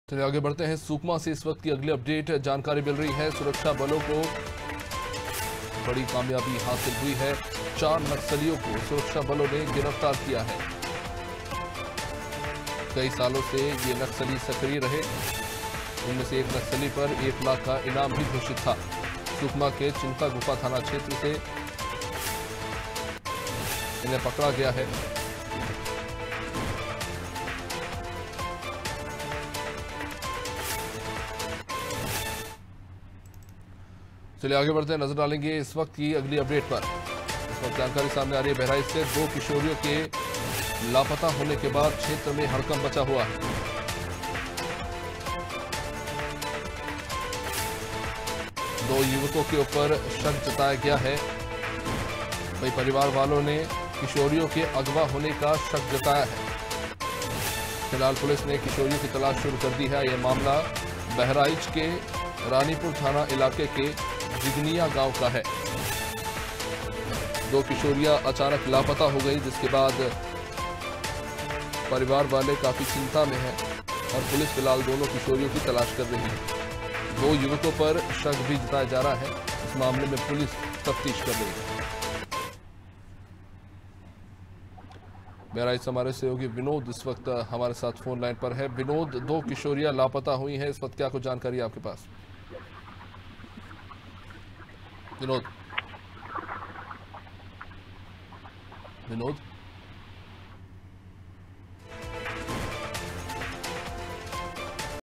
Sukma : सुरक्षाबलों को मिली बड़ी कामयाबी, चार नक्सलियों को किया गिरफ्तार || Anaadi Tv
सुकमा: सुरक्षाबलों को मिली बड़ी कामयाबी
चार नक्सलियों को किया गिरफ्तार
गिरफ्तार नक्सली कई वर्षों से थे सक्रिय
एक नक्सली पर एक लाख का इनाम था घोषित
सुकमा के चिंतागुफा थाना क्षेत्र का मामला
03 नक्सली आरोपी  2014 में कसालपाड मुठभेड़ जिसमें 14 जवान शहीद हुए थे उस घटना में  शामिल थे ।
गिरफ्तार सभी नक्सली नक्सल संगठन में विगत कई वर्षो से रहे है सक्रिय ।
आरोपियो की गिरफ्तारी में जिलाबल, एसटीएफ व 208 वाहिनी कोबरा, 150, 241 वाहिनी सीआरपीएफ कैम्प पिडमेल का योगदान रहा ।

COMING SOON
 DEN, AIRTEL